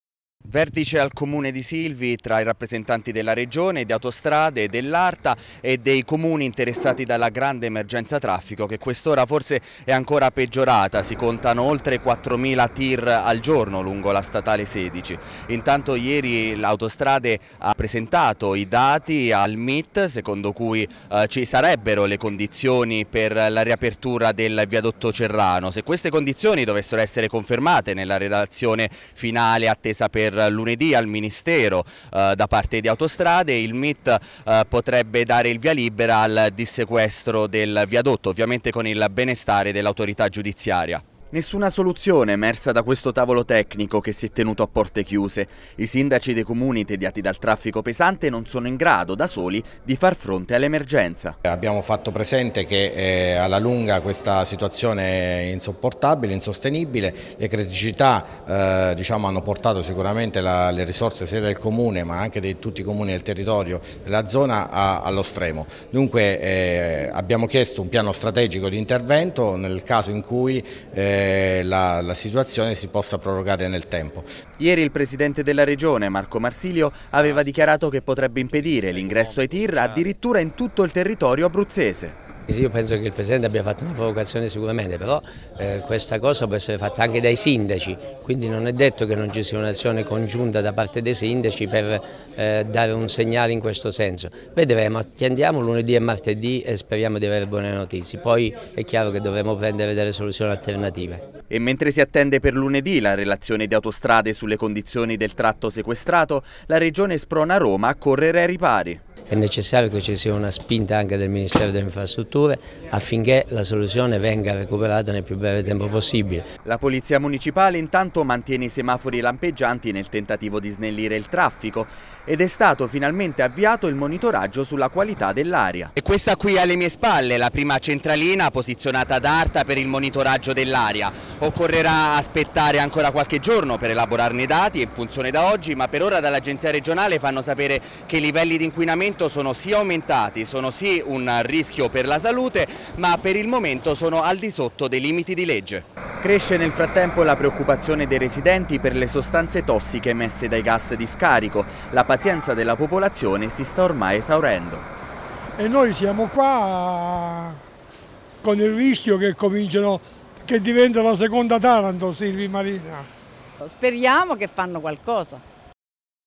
Emergenza traffico. Arriva la prima centralina per il monitoraggio dell'aria
Nessun passo avanti è stato fatto questa mattina a Silvi al vertice sull'emergenza traffico a cui hanno partecipato Regione, Comuni, Autostrade, Arta e gli altri enti abruzzesi. E, mentre cresce il rischio per la salute pubblica, è da oggi in funzione la prima centralina dell'Arta per il monitoraggio della qualità dell'aria.